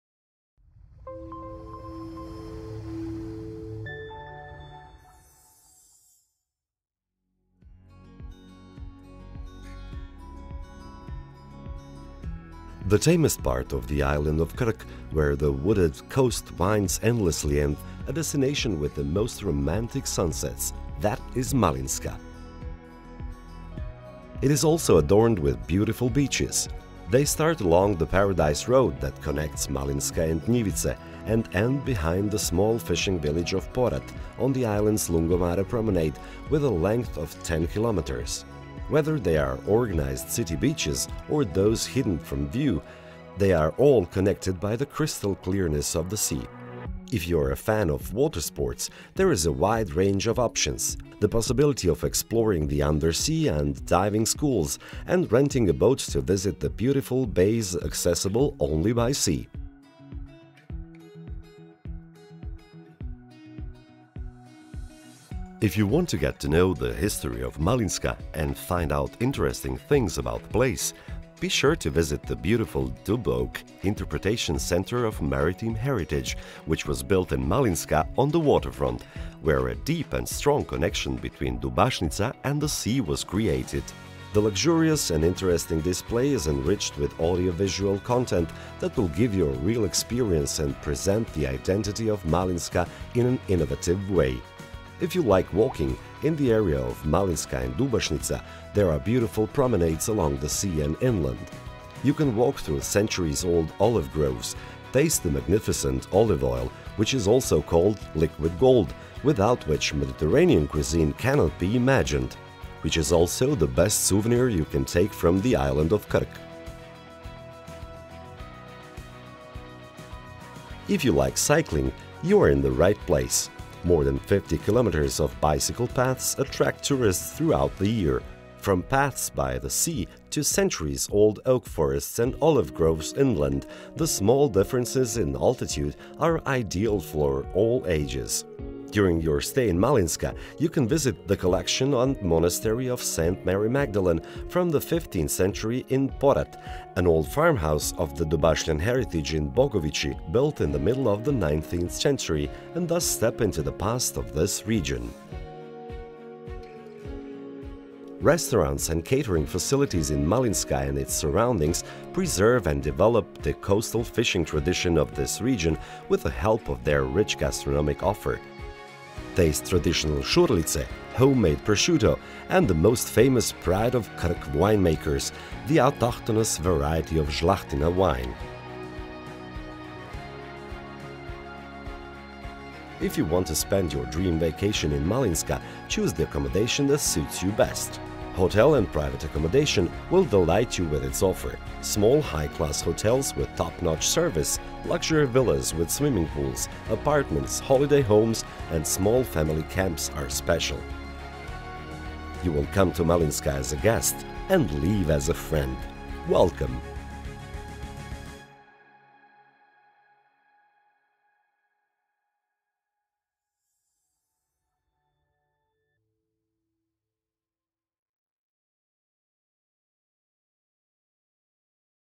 The Town of Malinska | Island of Krk | Croatia
Malinska is located in the northwestern part of the island, in the wide bay between the capes of Ćuf and Pelova.  Together with the twenty or so villages scattered around it, it forms the historic area of Dubašnica.  The most important natural resource of this region is the specific power and beauty of the sea and the forests of Dubašnica.

Malinska faces Kvarner Bay and Rijeka, Croatia’s largest port city. With its relatively low and accessible coast, the western part of Dubašnica offers spectacular views of Istria and Učka mountain.  Closer to the island’s center, on loose karst limestone soil and protected from strong winds, is the inland part of Malinska.  This location features one of the island’s most favorable climates and is rich with lush vegetation.